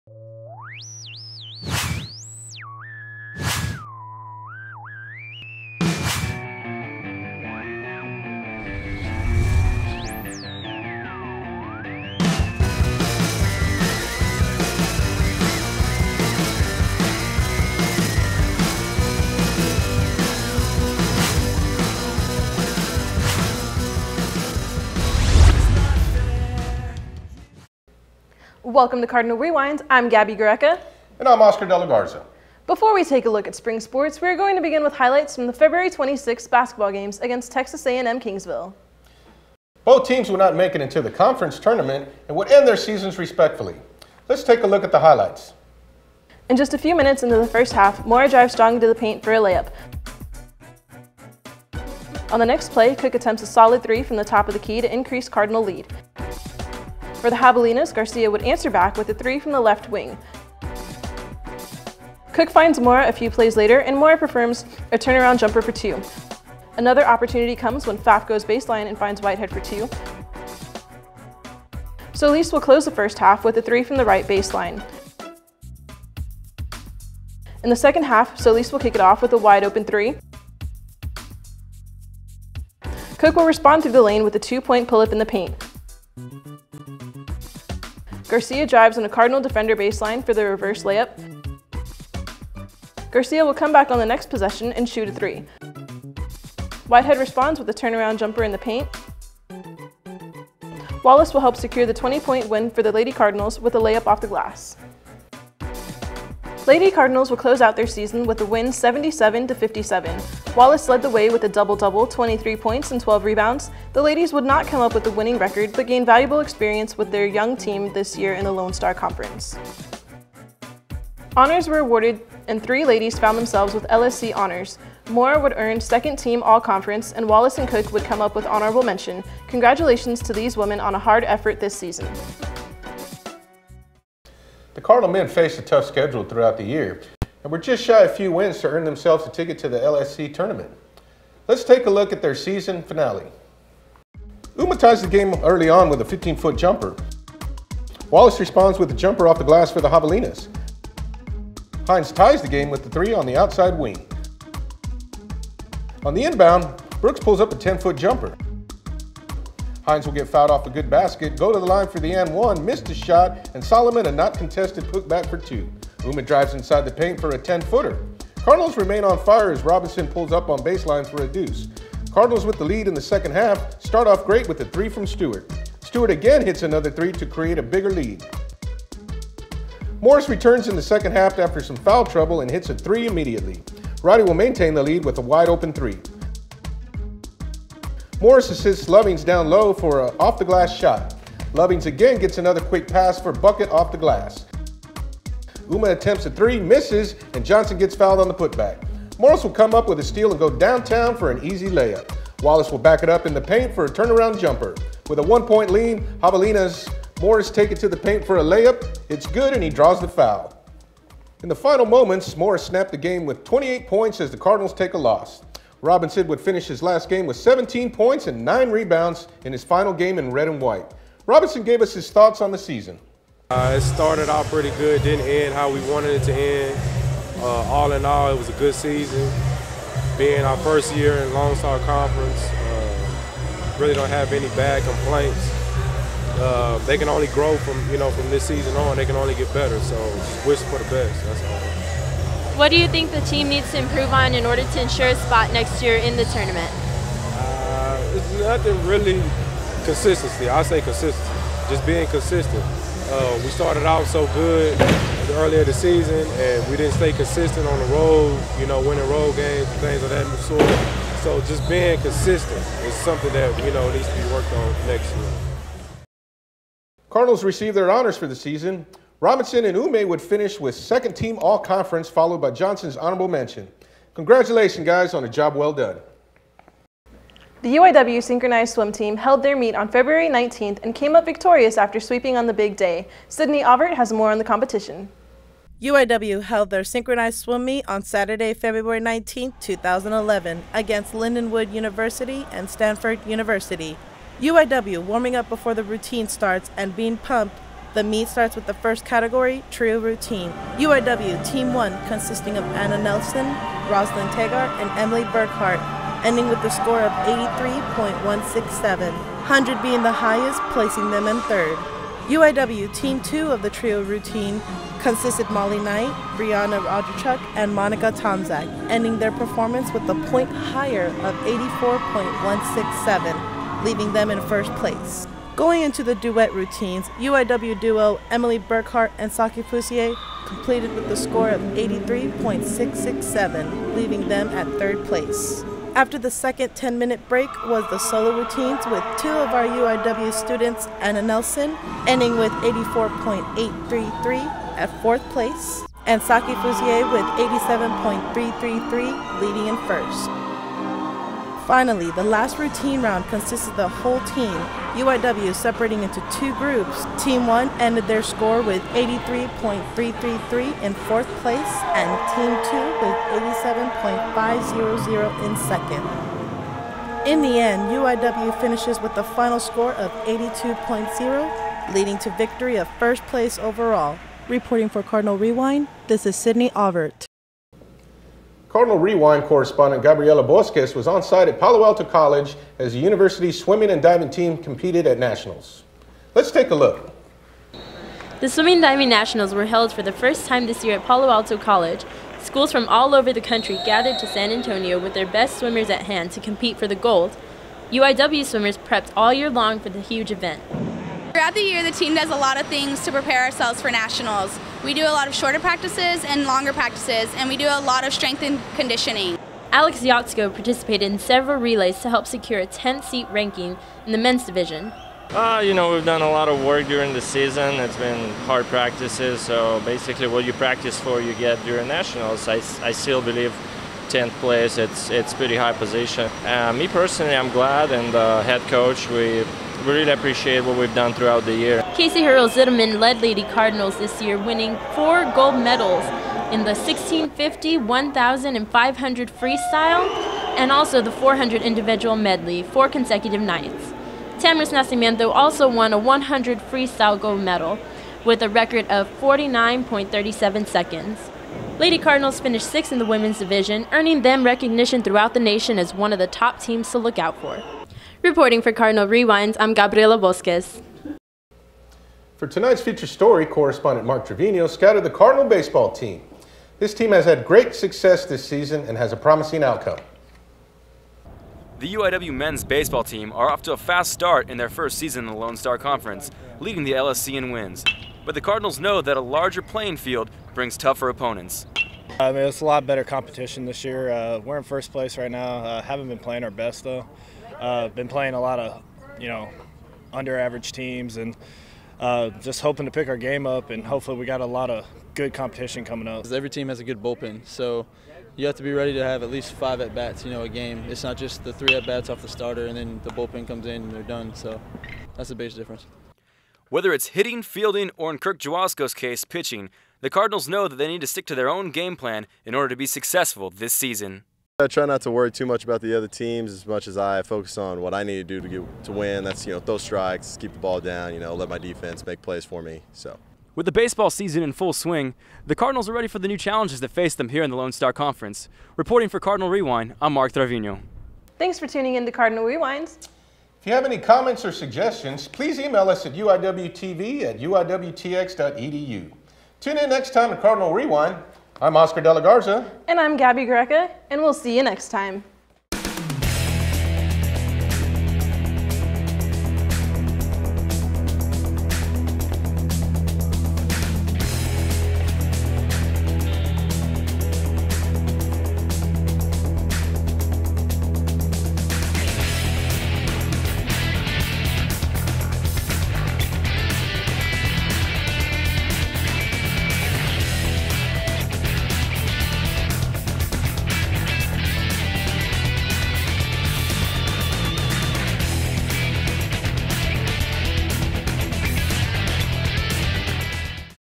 Cardinal Rewind April 2011
Let us take you inside the UIW Men's & Women's final basketball game against Texas A&M Kingsville. In this episode, we will highlight the women's synchronized swimming team, the national swimming event and take a look at men's baseball.